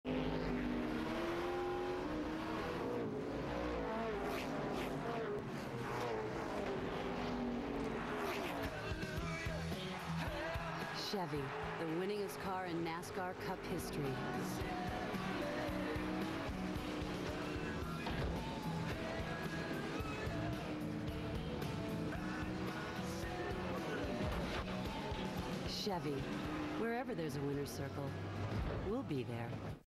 Chevrolet commercial - Jeff Gordon and Dale Earnhardt Jr to the finish line
Chevorlet commercial featuring Jeff Gordon and Dale Earnhardt, Jr. crossing the finish line and giving each other a side bump.